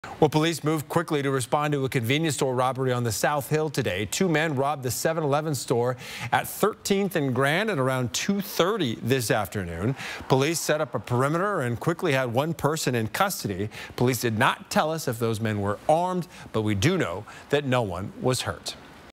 7-11 robbed in South Spokane
Two people robbed the 7-Eleven on South Grand Blvd. Monday afternoon.

According to the Spokane Police Department, two male suspects robbed the convenience store around 2:30 P.M. and fled on foot. SPD said they have one suspect in custody.

SPD could not confirm if either suspect was armed, but police confirmed no one was injured.